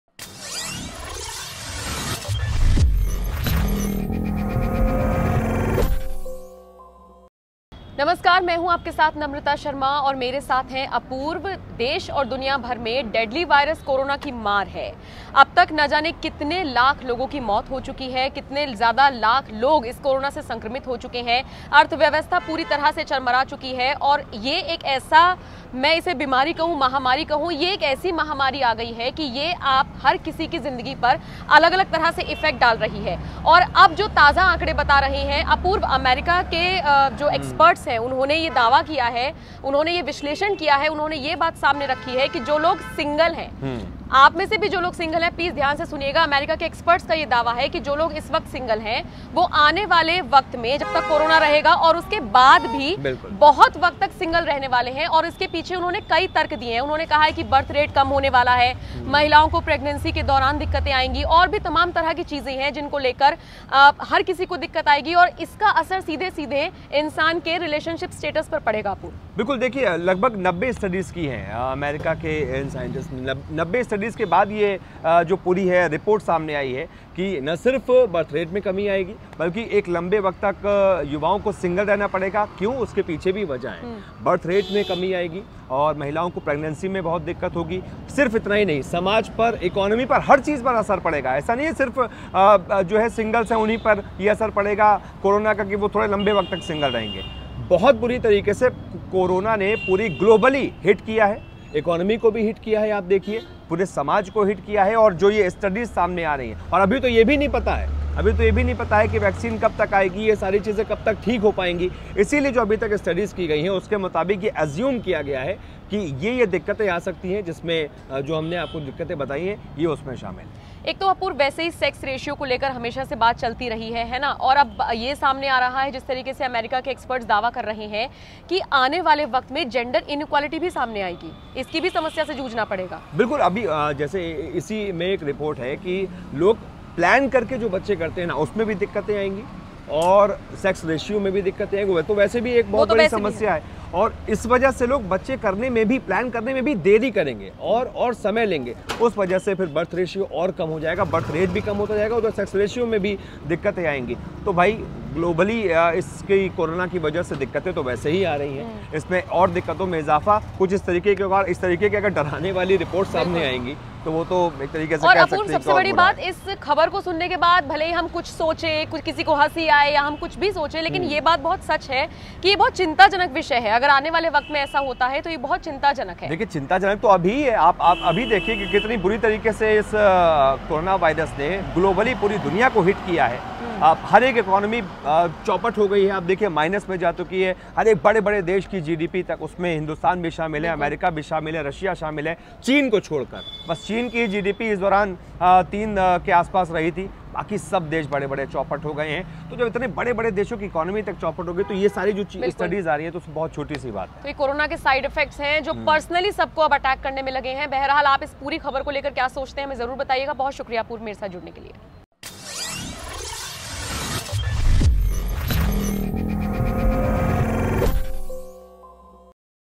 Covid की वजह से युवाओं को लंबे वक्त तक रहना होगा सिंगल
लंबे वक्त में कोरोना वायरस के प्रभाव को समझने के लिए अमेरिका के एक्सपर्ट्स ने करीब 90 स्टडी का विश्लेषण किया है. विश्लेषण के बाद एक्सपर्ट्स ने रिसर्च पेपर प्रकाशित किया है और चेतावनी दी है कि किस तरह से कोरोना वायरस समाज को बदल देगा. एक्सपर्ट्स का कहना है कि महामारी की वजह से युवाओं को अधिक वक्त तक सिंगल रहना होगा.